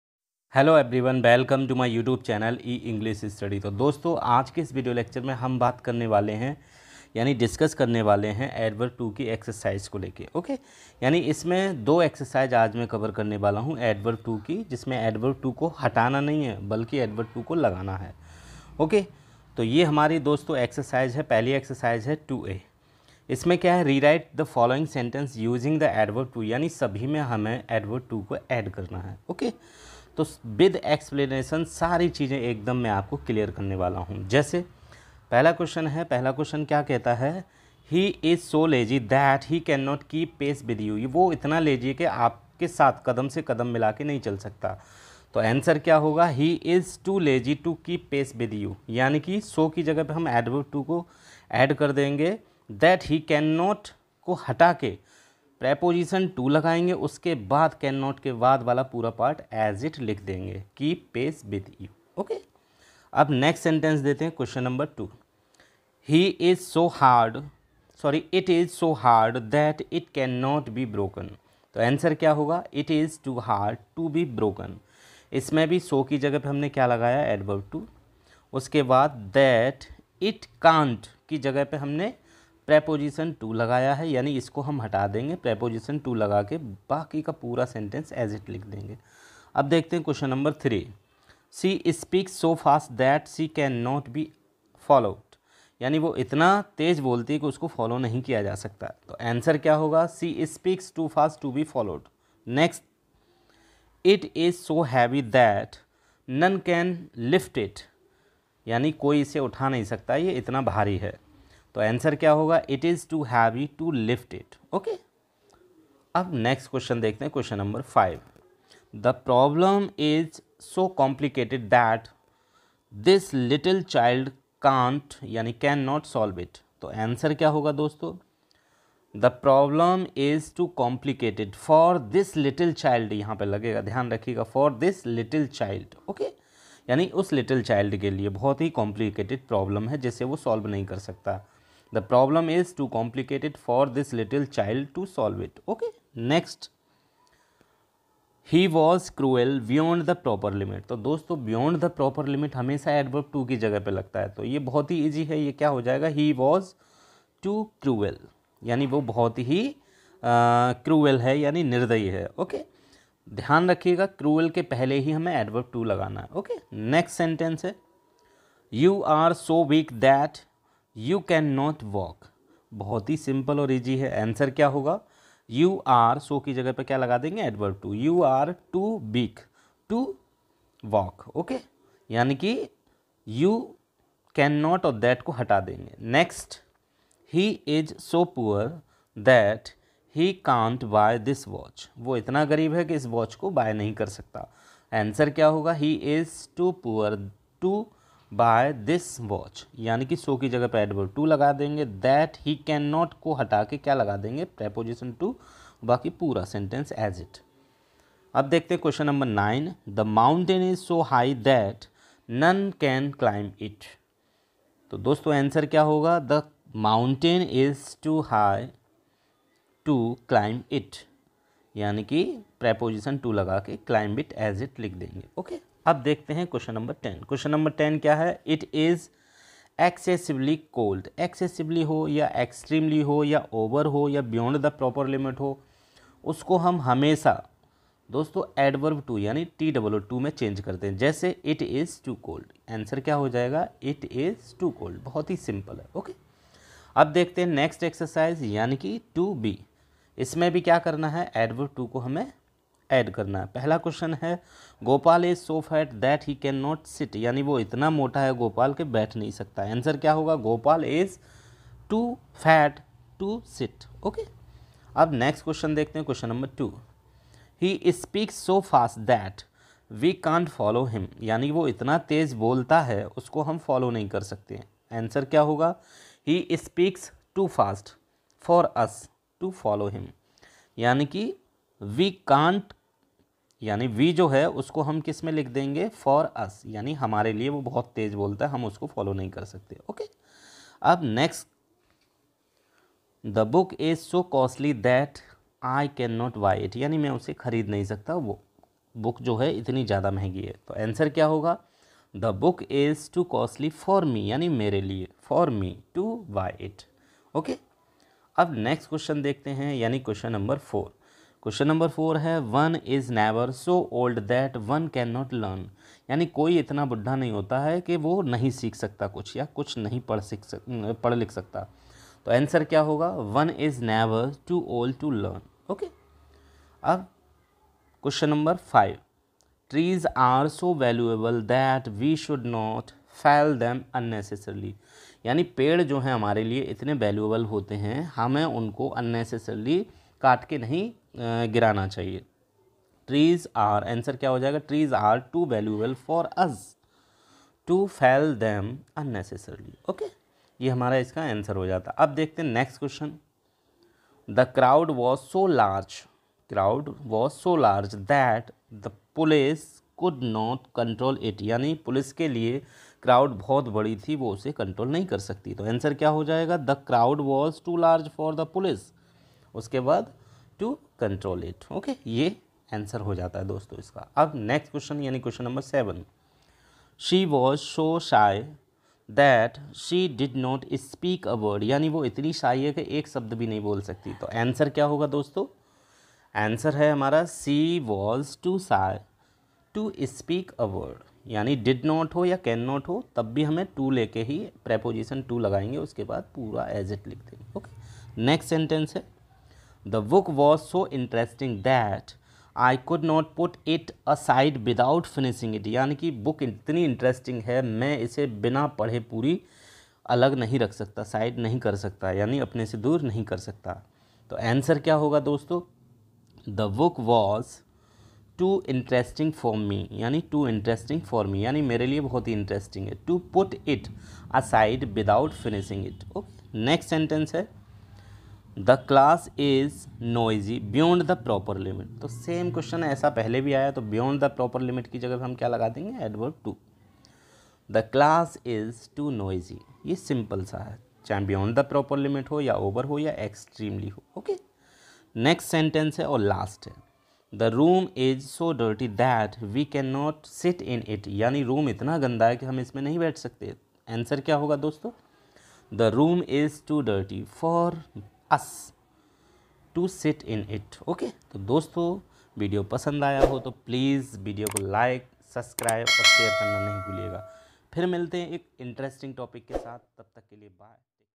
Solved Exercise of Adverb too|Exercise No.2A & 2B.
Adverb too solved exercise| part-2| Up Board grammar exercise for class 11th and 12th|

Hello friends !
'e English study'.
In this video lecture, I'll solve two exercise of adverb 'too' exercise 2A and 2B.
These two exercises are for class 11 and 12.
Students of 11th and 12th class can get complete solution with  explanation.
      and
Thanks a lot my dear friends to support me.
If you want any special video on any topic, you can comment me on comment section.
Links :-

Adverb too solved exercise| Part 1| exercise 1A & 1B| UP board English grammar exercise for 11 &12.